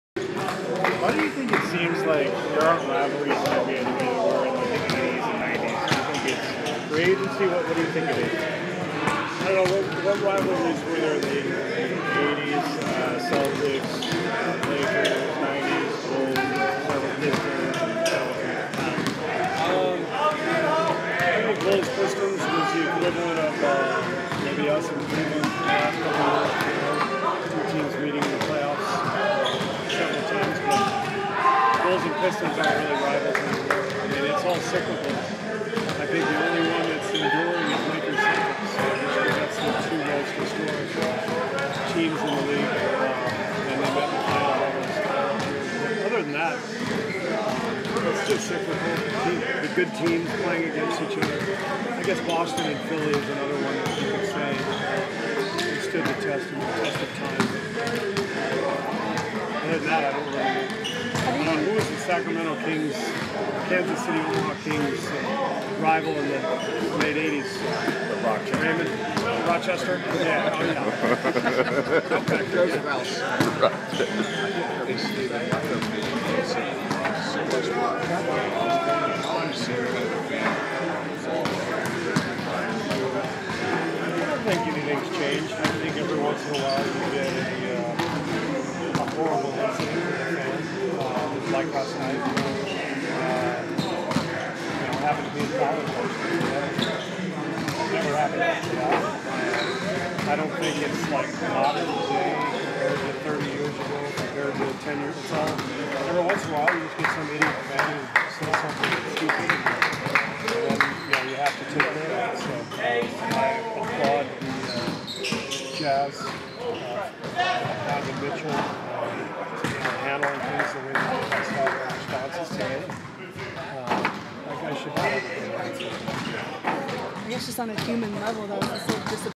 Kerr on old NBA rivalries; on the Westbrook altercation says it’s always been one fan ruining it
From Golden State Warriors () pregame-morning shootaround-practice-postgame before-after opponent, at Rakuten in Oakland,